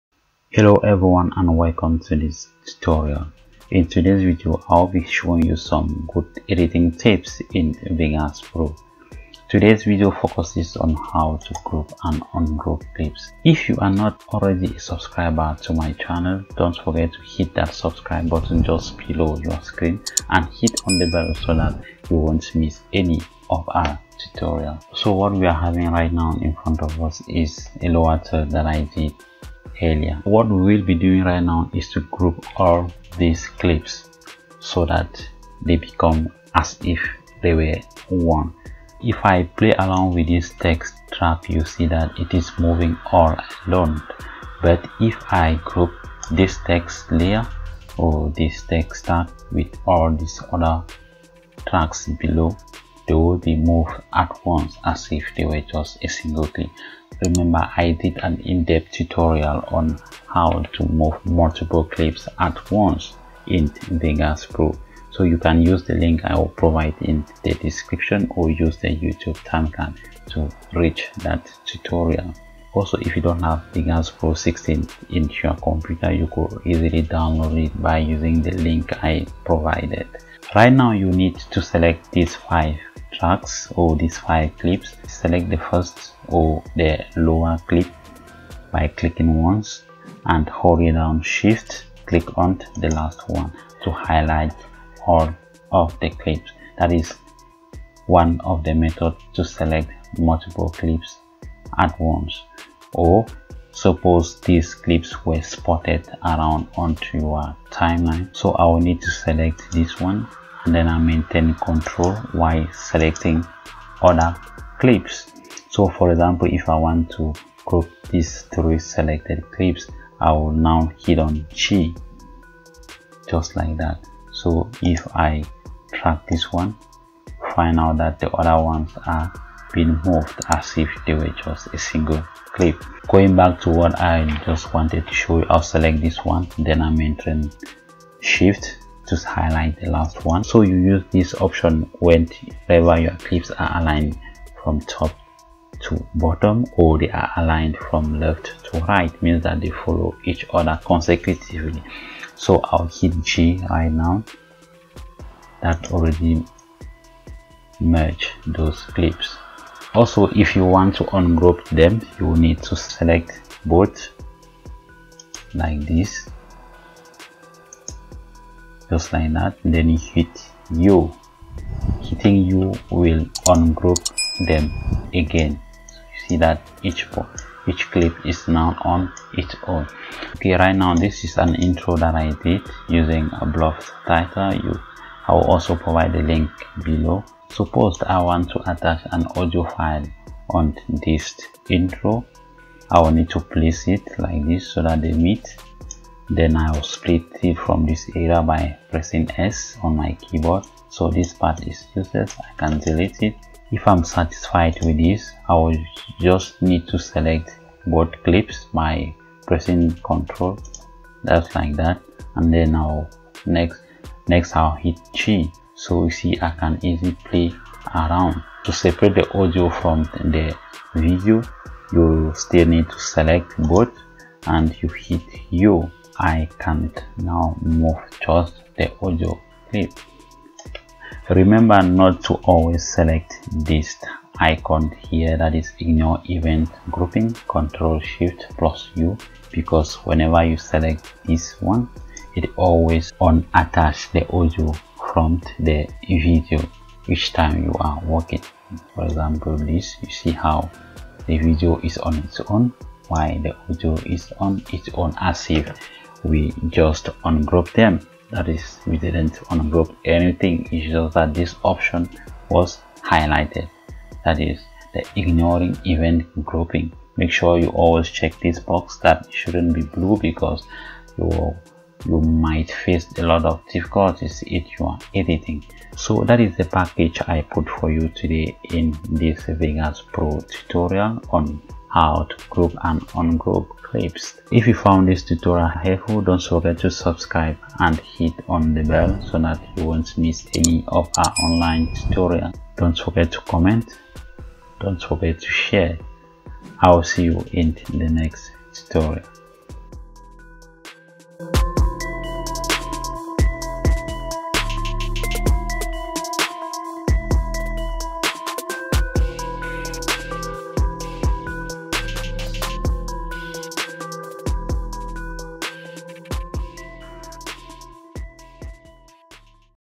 How to Group and Ungroup Clips/Items in Vegas Pro
Hello guys,
In this video i will be showing you some quick editing tips in Vegas Pro. Knowing how to group and ungroup items or clips is very crucial in video editing.

Use the following commands to group, ungroup or to split clips in Vegas Pro,
Press G for Grouping, Press U for Ungroupping, Press to Split Clips

NB/ make sure not to select the option  Ignore Event Grouping-Ctrl+Shift+U as this automatically ungroup items which may slow down your editing

Whatsapp and Line: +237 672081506